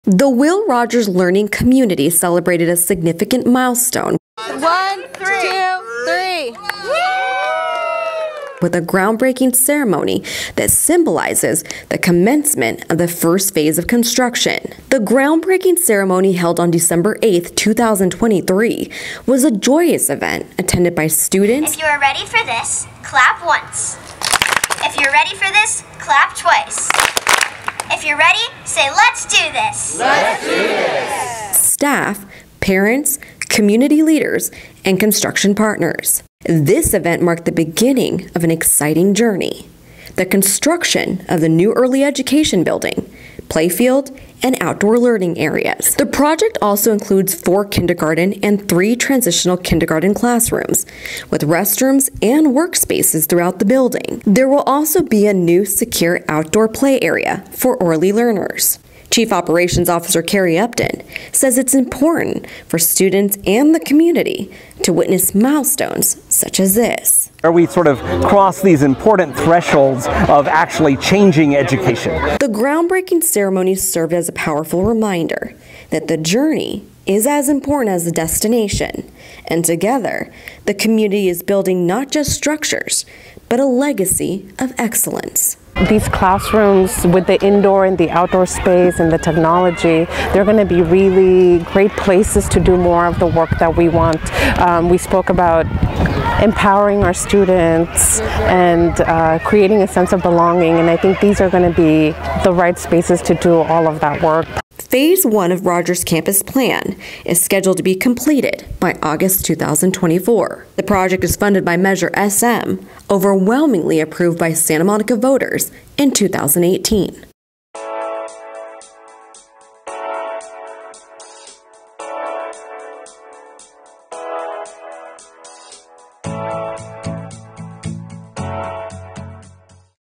Will Rogers Learning Community Groundbreaking Ceremony Celebrates First Phase of Construction
To celebrate the start of construction on the new early education building, playfield and outdoor learning areas, Will Rogers Learning Community hosted its groundbreaking ceremony on Friday, Dec. 8, 2023. The Santa
Monica Malibu Unified School District’s (SMMUSD) Board of Education,
superintendent, WRLC principal, SMMUSD staff, local elected officials, community members, students and building partners attended the celebration.